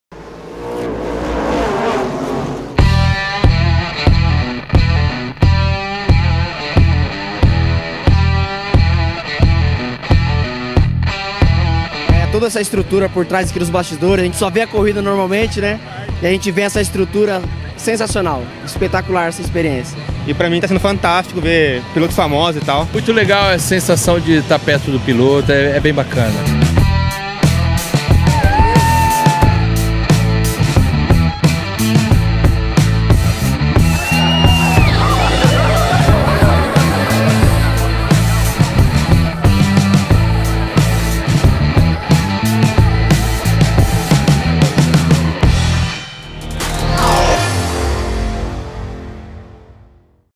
STOCK CAR 2019 | GRID EXPERIENCE, o ingresso que coloca você no grid da Stock Car
Uma experiência única e inesquecível para o amante da velocidade, o Grid Experience te coloca literalmente no grid de largada a poucos minutos da corrida, com direito a conhecer o safety car e encontrar os pilotos antes que os motores dos carros sejam ligados. E você ainda tem direito a uma área exclusiva para assistir ao pódio!

As vagas são limitadas. Garanta a sua acessando

A nona etapa da Stock Car 2019 vai ser em Cascavel, um dos palcos mais tradicionais da categoria, que em 2019 completa 40 anos de velocidade. Vá ao Autódromo Zilmar Beux e faça parte dessa festa!

Duas provas cheias de ultrapassagens e emoção na pista mais veloz do calendário brasileiro, muitas atrações de entretenimento no Village e brindes das equipes e dos pilotos na visitação aos boxes. Tudo isso te espera neste domingo, em um evento para todas as idades.